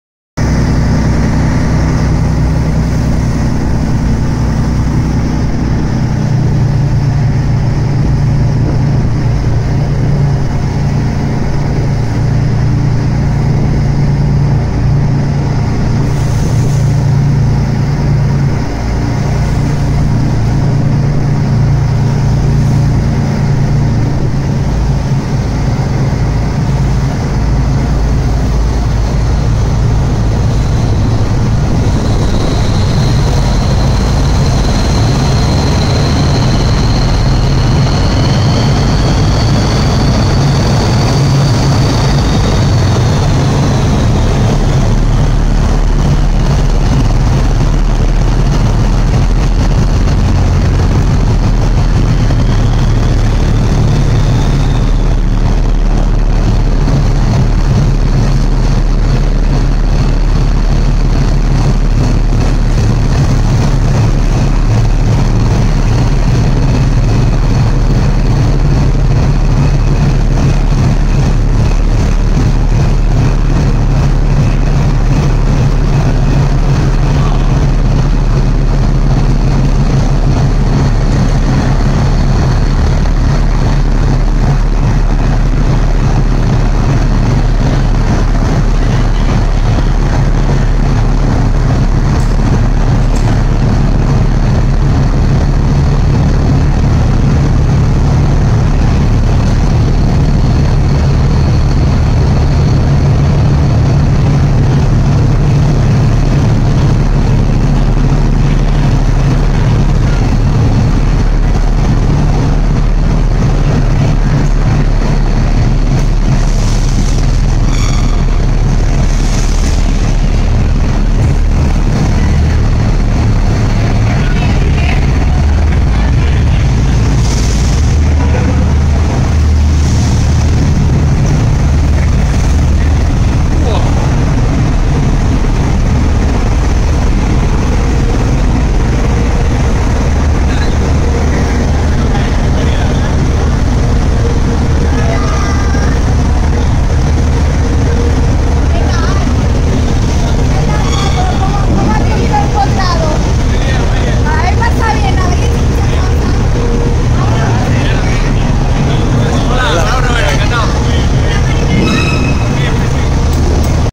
Llegando al buque Galicia
En LCM entrando en el dique del buque Castilla.